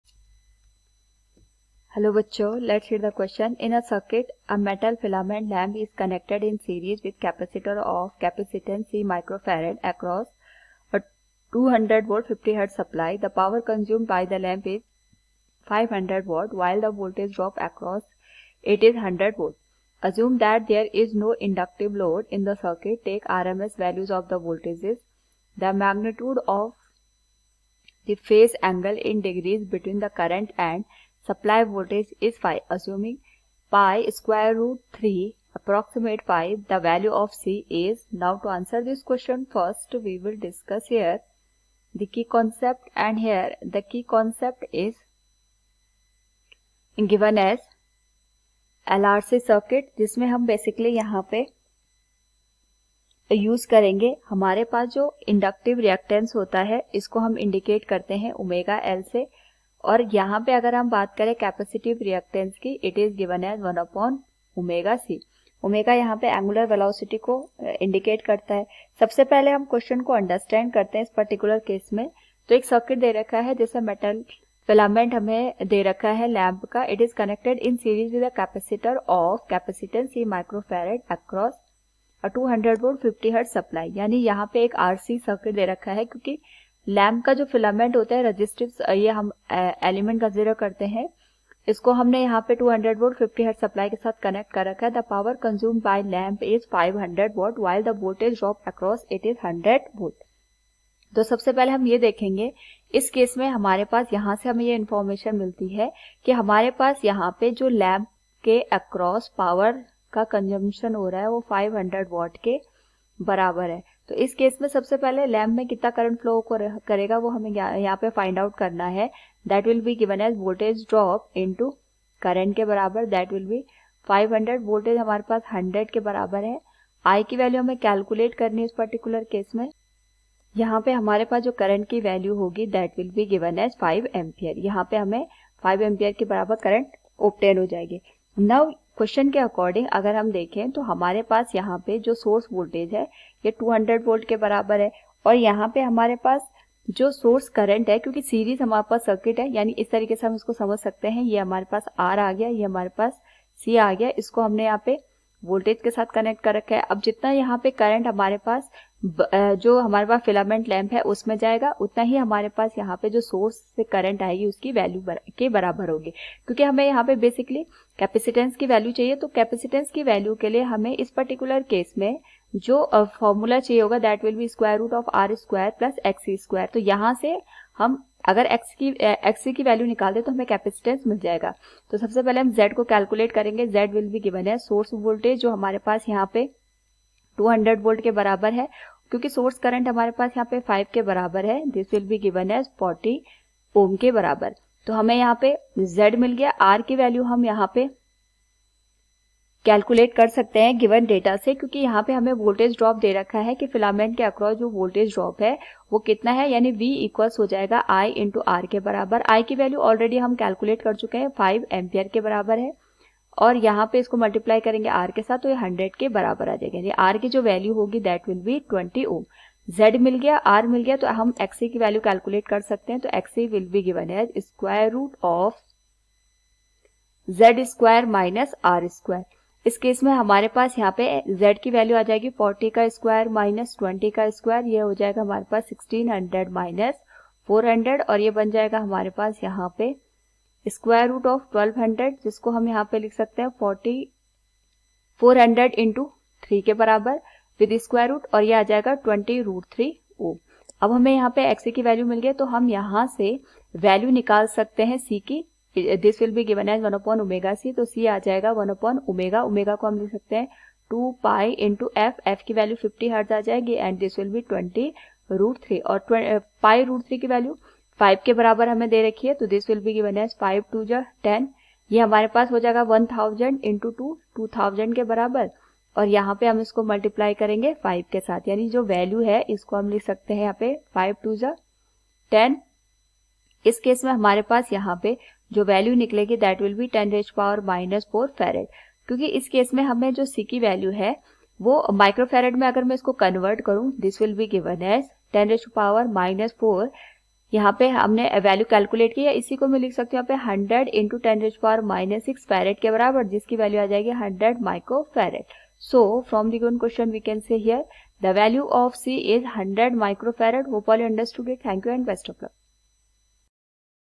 In a circuit, a metal filament lamp is connected in series with a capacitor of capacitance \\(\\....
In a circuit, a metal filament lamp is connected in series with a capacitor of capacitance \\(\\mathrm{C} \\mu \\mathrm{F}\\) across a \\(200 \\mathrm{~V}, 50 \\mathrm{~Hz}\\) supply. The power consumed by the lamp is \\(500 \\mathrm{~W}\\) while the voltage drop across it is \\(100 \\mathrm{~V}\\). Assume that there is no inductive load in the circuit. Take rms values of the voltages. The magnitude of the phase angle (in degrees) between the current and the supply voltage is \\(\\varphi\\). Assume, \\(\\pi \\sqrt{3} \\approx 5\\).